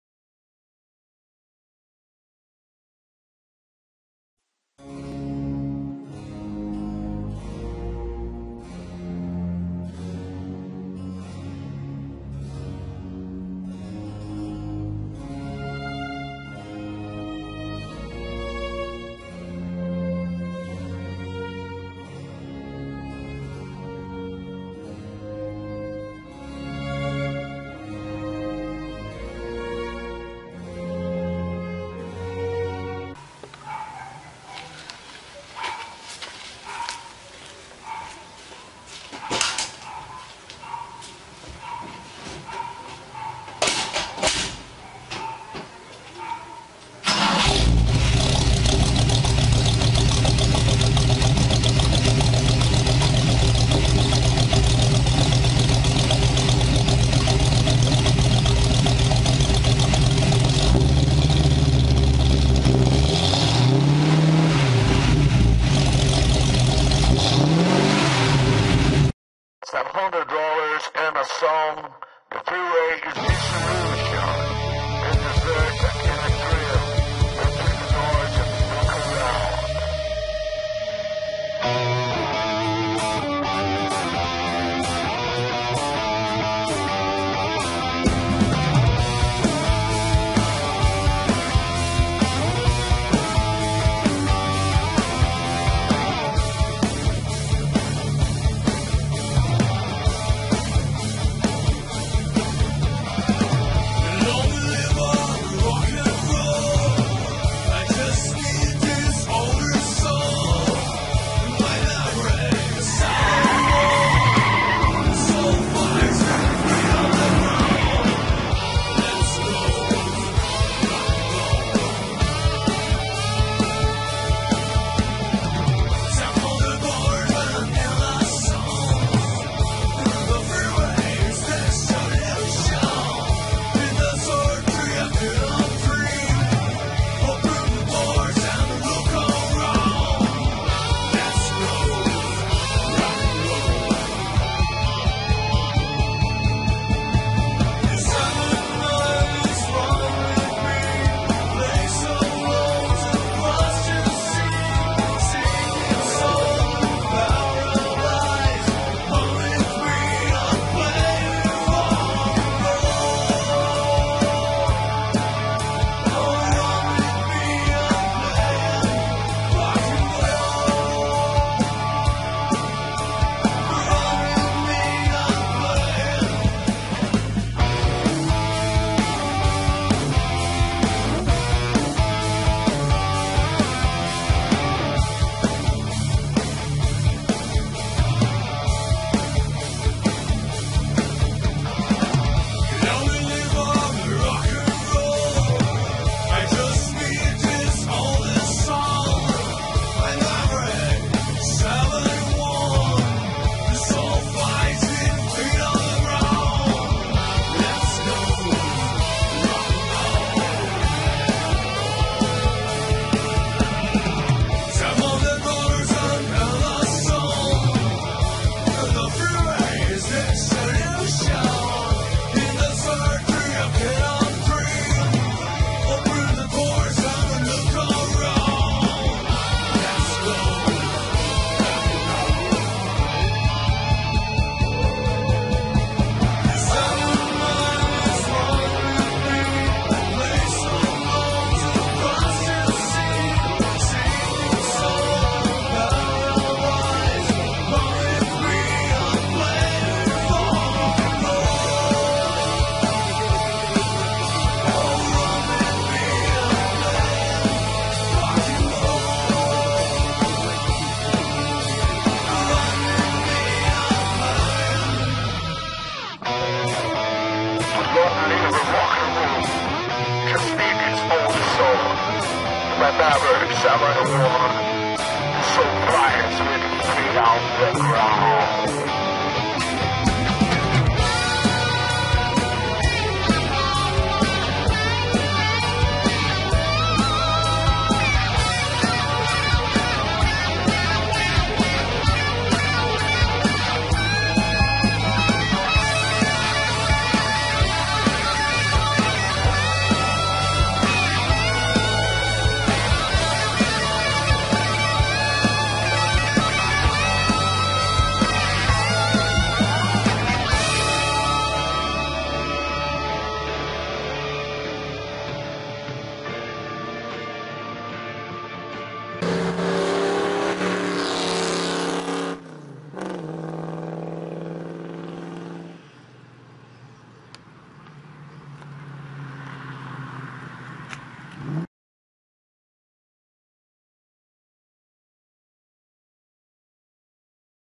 FITTIPALDI E O MAVERICK - O DIA QUE A LENDA DOMOU A FERA
Emerson Fittipaldi encarou o Ford Maverick no autódromo em uma época onde a vida era mais apaixonante para o automobilismo...

Se inscrevam !!